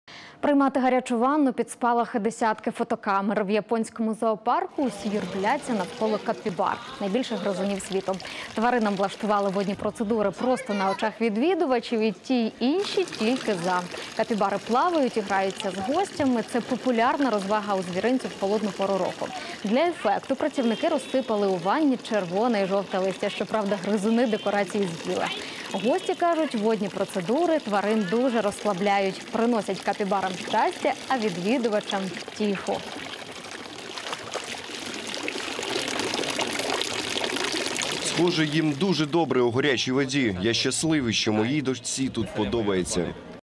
У Японському зоопарку капібарам влаштували водні процедури на очах відвідувачів
І ті й інші тільки за. Капібари плавають і граються з гостями.  Це –  популярна розвага у звіринцю в холодну пору року.

Для ефекту працівники  розсипали у ванні червоне і жовте листя. Щоправда, гризуни декорації з'їли. Гості кажуть, водні процедури тварин дуже розслабляють. Приносять капібарам щастя, а відвідувачам втіху.